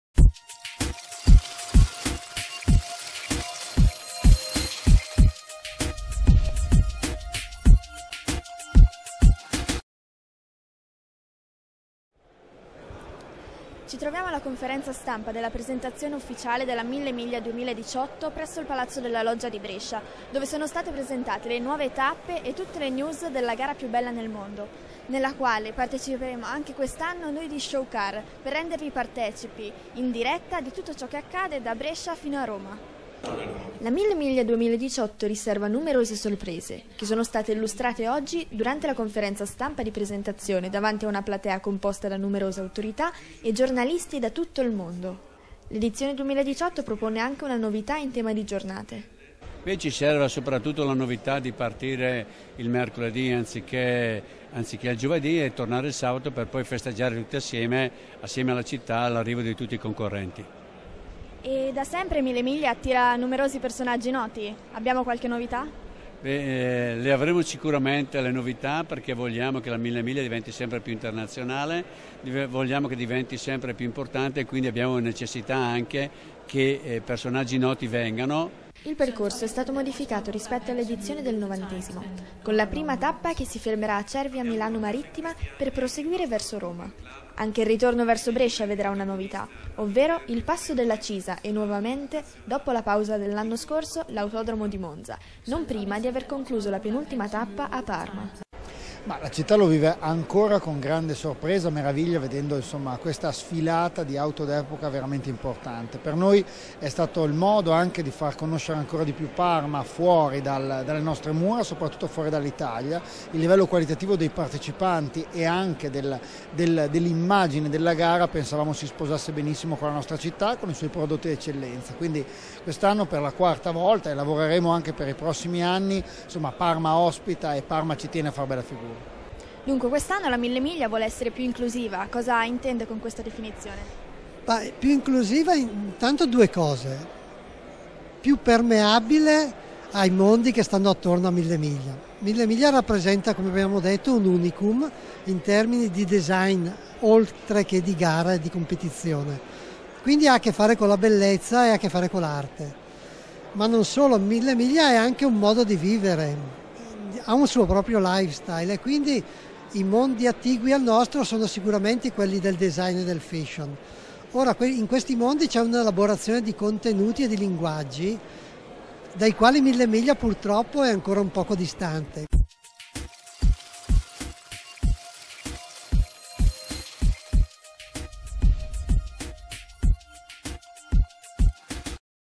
Mille Miglia 2018 Conferenza stampa
La Conferenza stampa di presentazione della Mille Miglia 2018, con numerose novità, sia per quanto concerne le tappe che i giorni di gara.